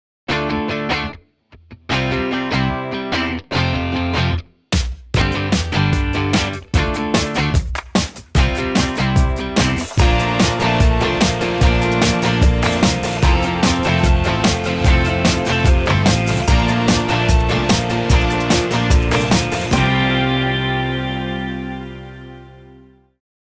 Miele CM6 vrijstaande koffiemachines met OneTouch for Two | De Schouw Witgoed
Ontdek de handige OneTouch for Two functie. Met één druk op de knop tegelijkertijd twee koppen koffie zetten.